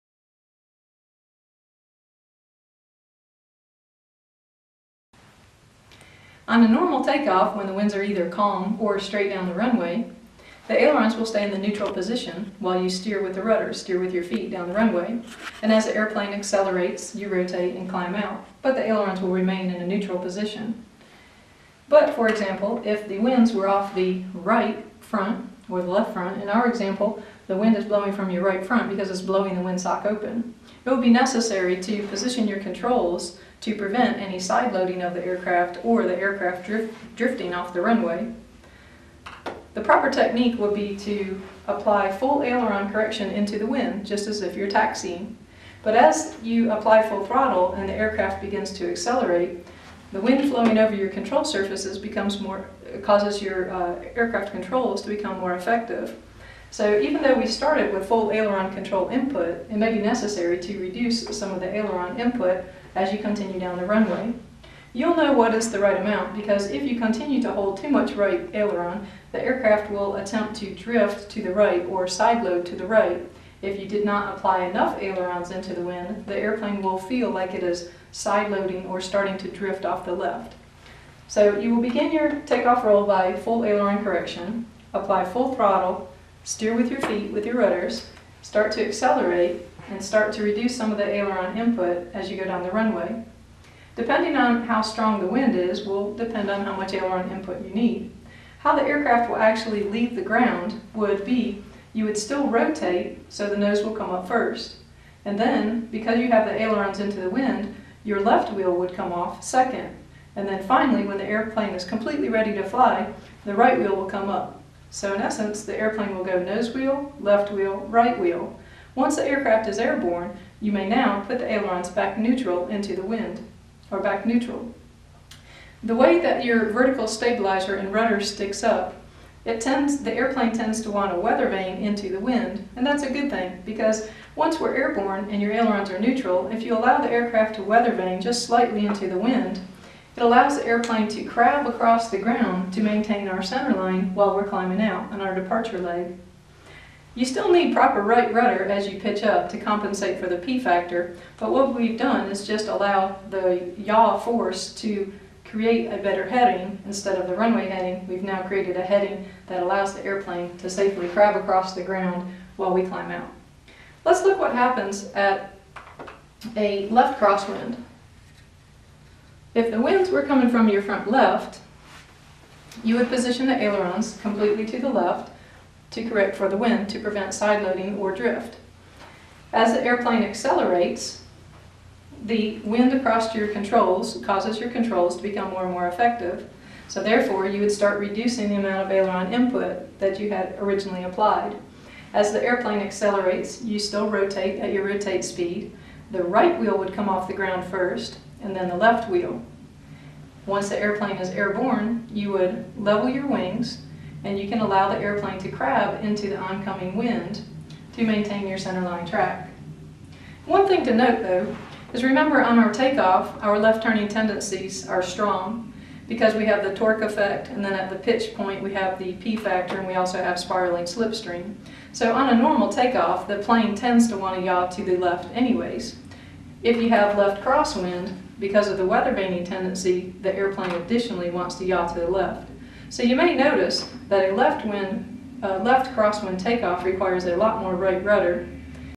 Crosswind Takeoff (Private Pilot Lesson 3e)
Explanation of how to properly takeoff with crosswind conditions.

Thinking of becoming a pilot?

As per request, new videos with better audio and video quality will be coming soon!